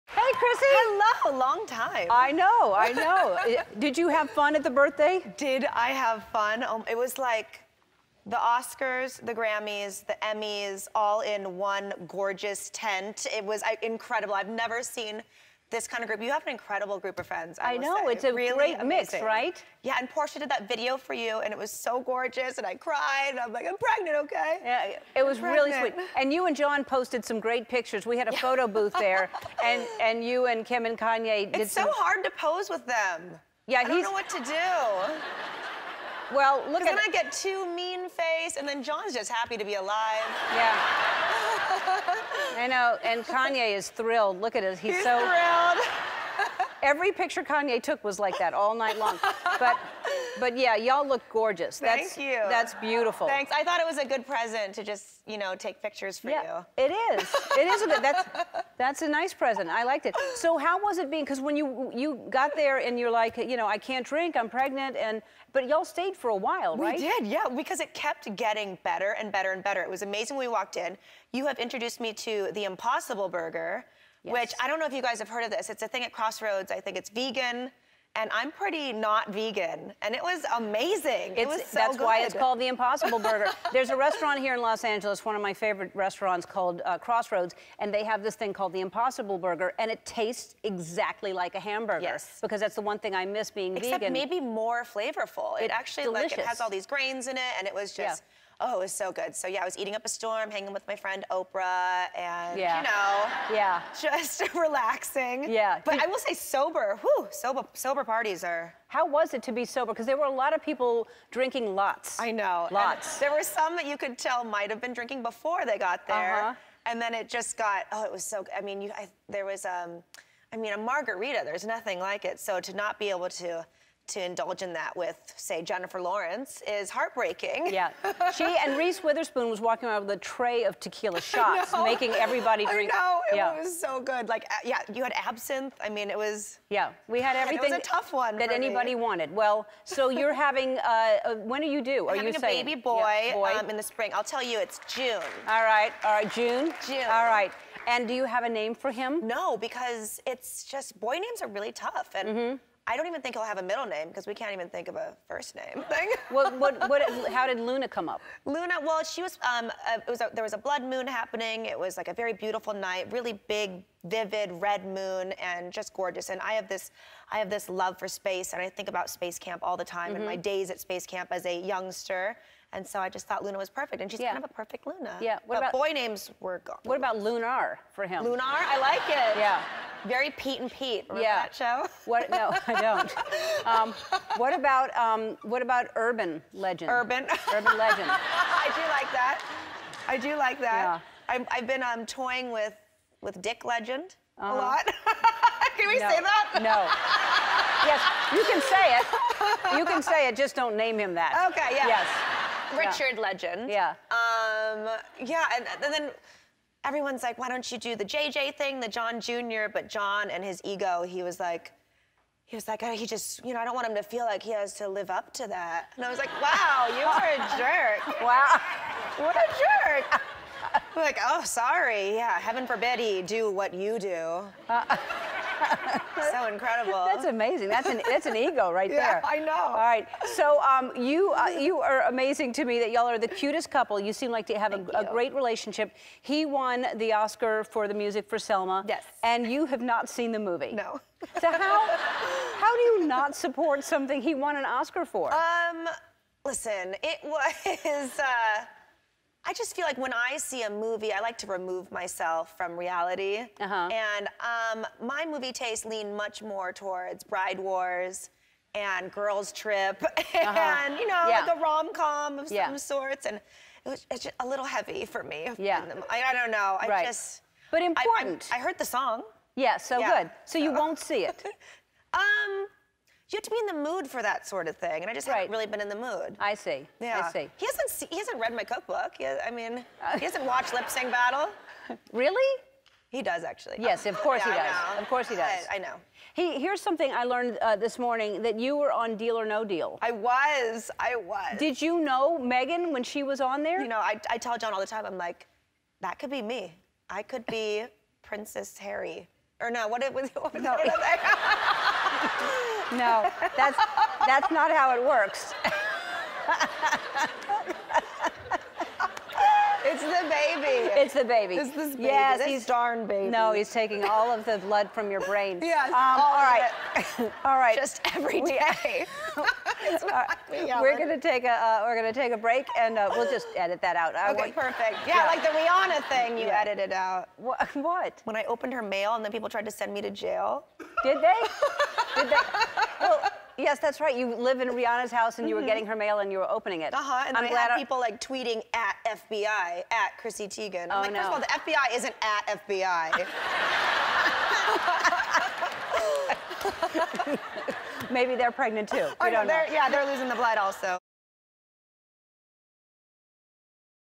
Chrissy Teigen is Unsure on How to Pose in Photos with Kanye West
Cookbook author Chrissy Teigen is good friends with Kim Kardashian and Kanye West, but she was at a loss as to how to pose with the serious rapper in Ellen's birthday party photo booth. Plus, the model talks possible names for her son with John Legend, and being on "Deal or No Deal" with future royal Meghan Markle.